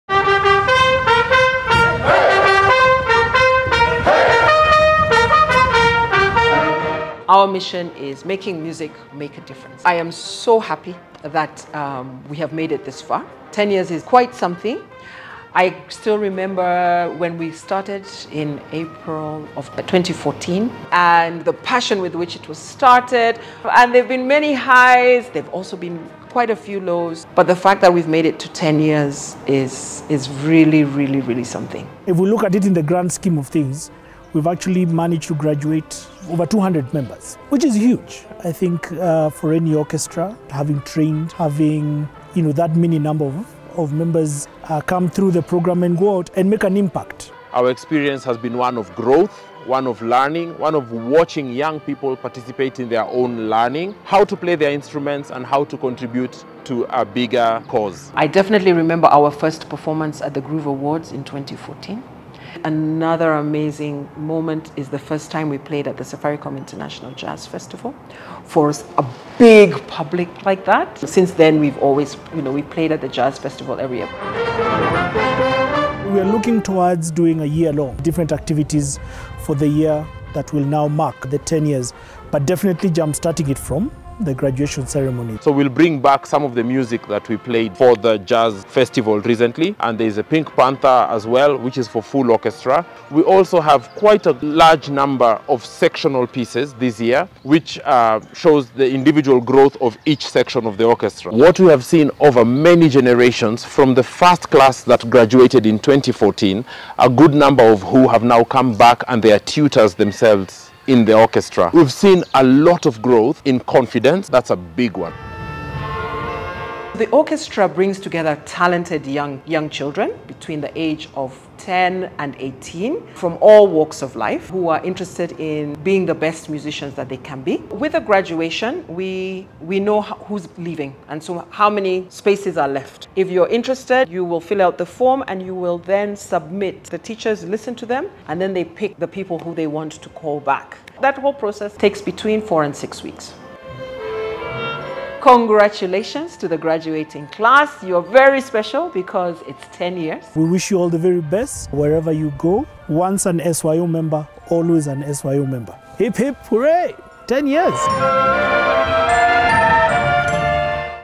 Safaricom Youth Orchestra | Celebrating 10 Years of Musical Excellence
Celebrating 10 Years of Musical Excellence with the Safaricom Youth Orchestra! We're not just celebrating a graduation ceremony, we're commemorating a decade of nurturing young musical talent in Kenya!  Over the years, the Safaricom Youth Orchestra has seen over 200 graduands, graced the stage at the annual Safaricom Jazz Festival, and witnessed inspiring alumni return as tutors. ✨

Join us in celebrating this incredible milestone and the future of Kenyan music! 🇰🇪 SafaricomYouthOrchestra.